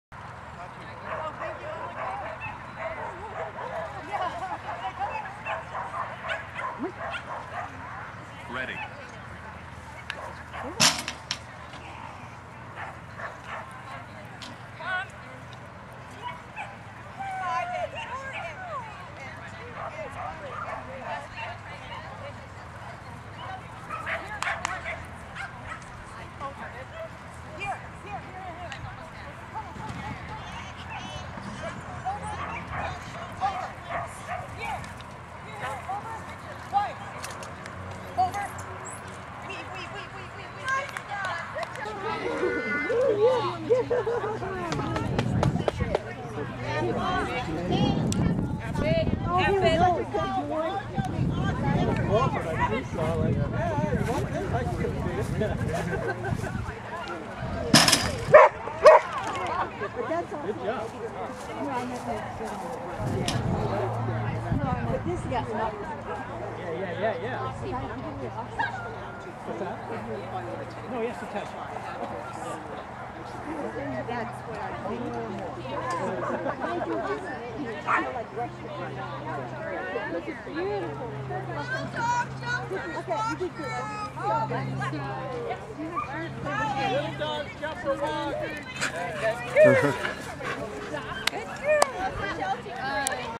Avid's Standard Runs August 25, 26 2018
Woring on our contact criteria with the baby dog, Avid always tries so hard and is such a good girl.